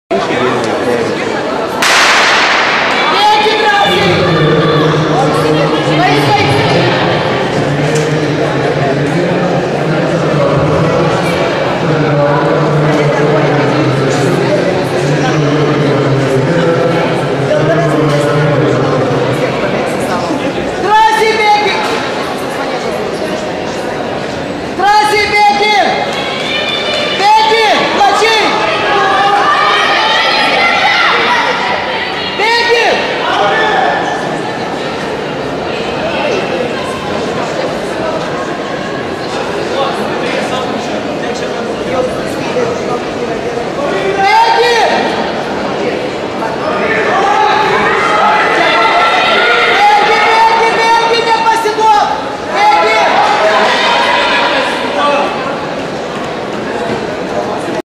Lietuvos žiemos čempionatas 2013. 400m 1 bėgimas. Vyrai
1 Ignas Dailidėnas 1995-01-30 Panevėžys PKKSC "Žvaigždė" 53.47
2 Laurynas Gedvilas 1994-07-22 Jonava KKSC  53.78
3 Gediminas Uždavinys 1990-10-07 Vilnius VU  54.19